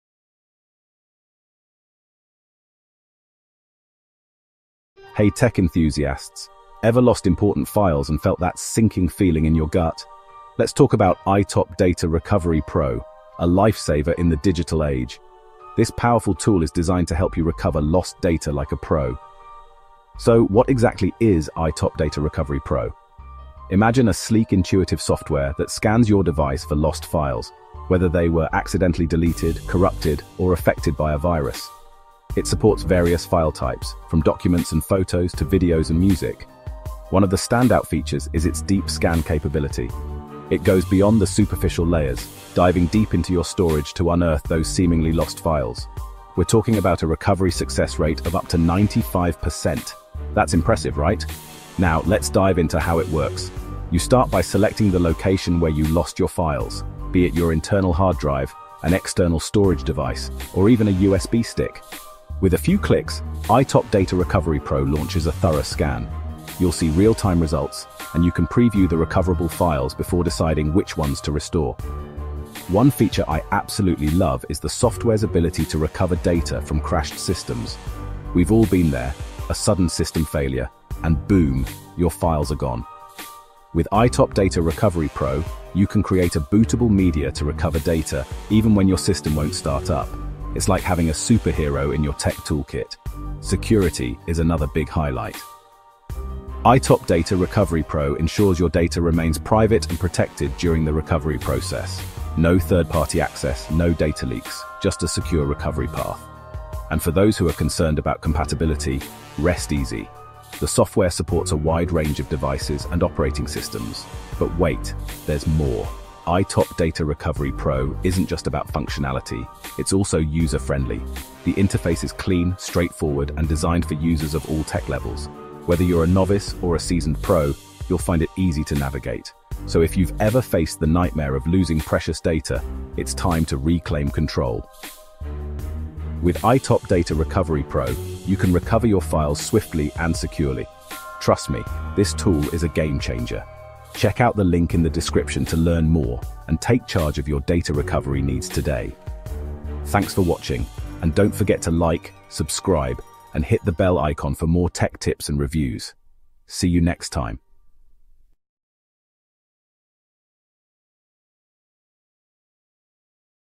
🔄 iTop Data Recovery Pro - Pulihkan File yang Terhapus dengan Mudah & Cepat!
Apakah Anda kehilangan file penting karena terhapus atau terformat? 🔥 Jangan panik! iTop Data Recovery Pro hadir untuk membantu Anda memulihkan file yang hilang dengan cepat dan efisien. Dengan fitur pemulihan canggih, software ini dapat mengembalikan dokumen, foto, video, dan berbagai file lainnya hanya dalam beberapa klik. 💻⚡

✅ Pemulihan cepat & akurat
✅ Mendukung lebih dari 1000 format file
✅ Kompatibel dengan HDD, SSD, USB, dan kartu memori

🎯 Tonton video ini untuk mengetahui cara menggunakan iTop Data Recovery Pro dan selamatkan file Anda sekarang!

🔑 428D9-5659D-2FAE7-EB9B3
🔑 F1E12-26334-BE863-C35B5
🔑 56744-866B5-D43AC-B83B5
🔑 5F653-D8647-F43B8-A2945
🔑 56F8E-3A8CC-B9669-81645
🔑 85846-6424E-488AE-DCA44
🔑 E2242-6B229-83D4F-421B4